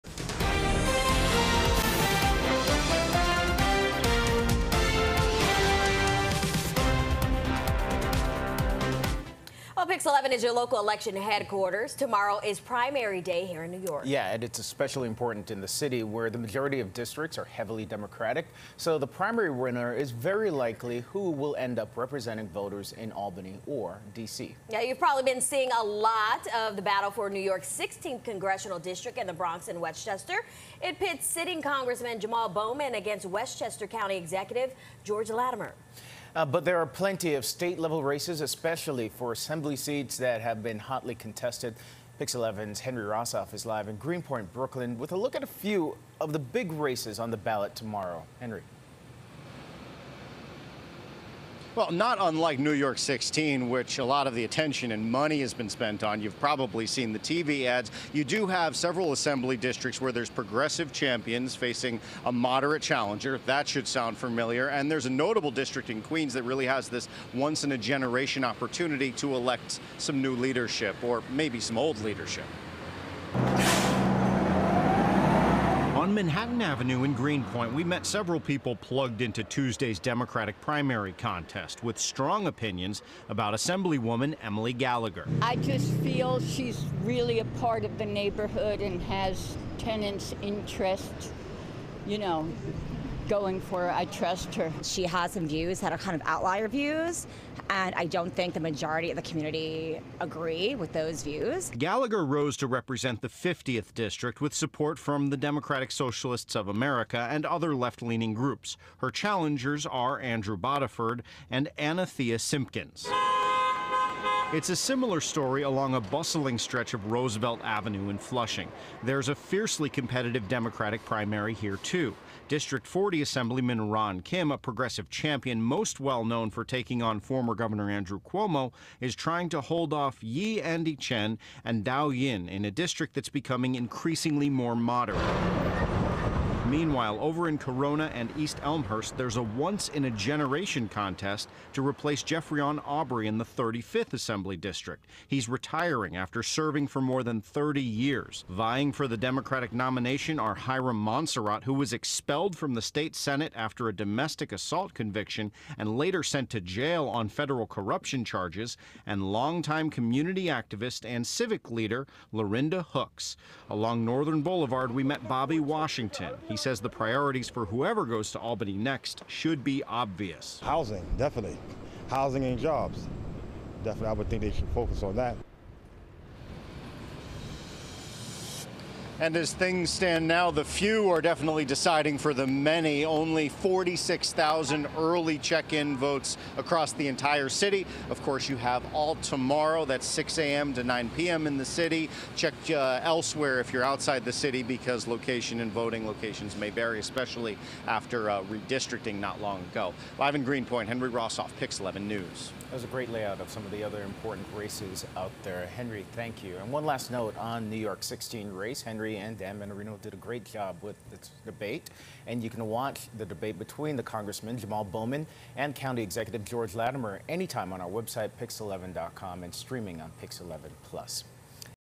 Closely contested races on the ballot to watch on Primary Election Day
While New York's 16th Congressional District in the Bronx and Westchester has gotten much of the attention, there are other closely contested races-- particularly at the State Assembly level.